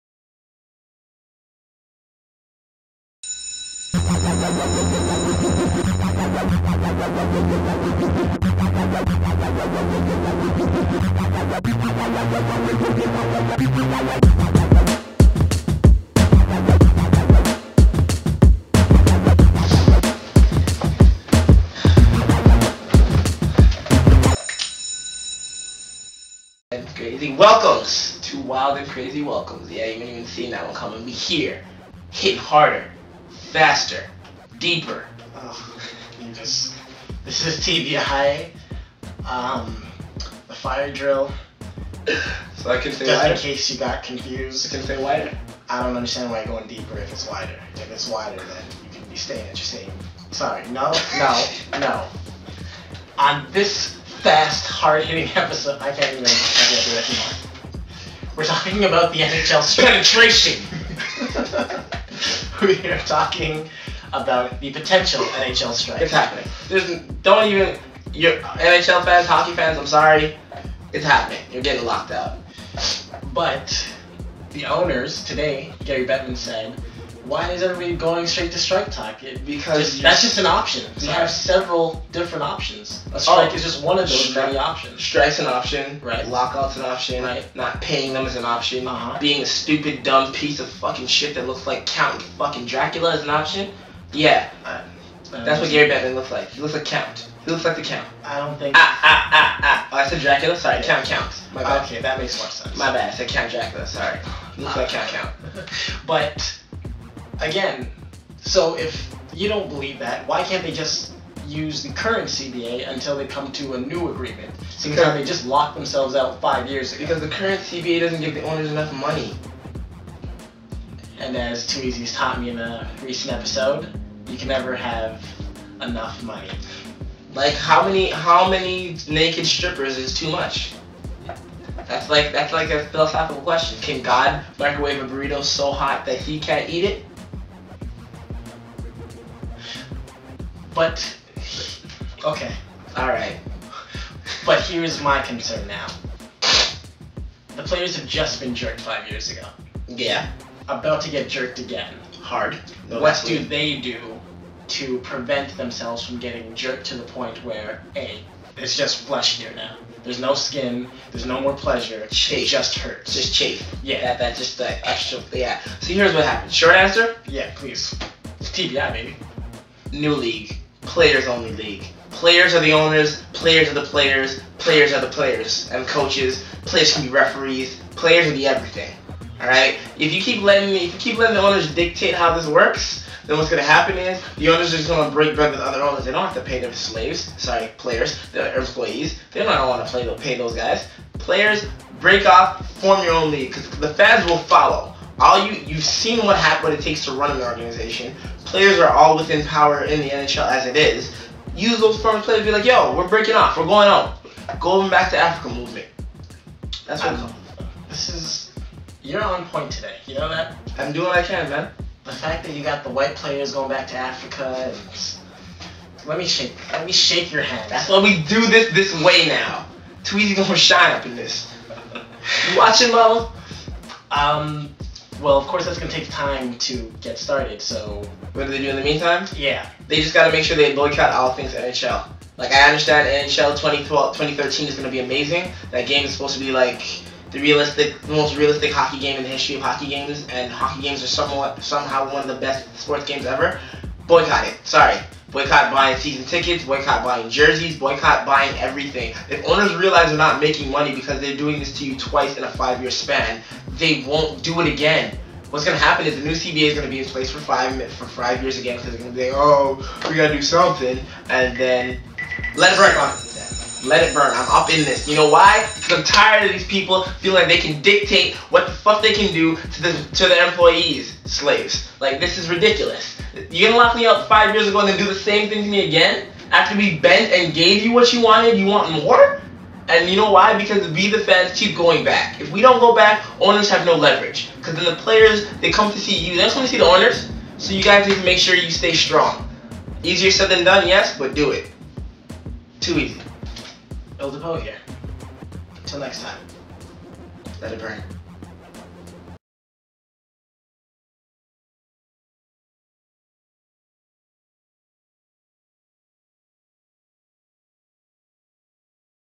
TBI Fire Drill - NHL Lockout
Deja Vu much? Several years ago the NHL locked out the league and all its (slaves) players for an entire season and ill Dapoe & 2 Easy share their take on this and as always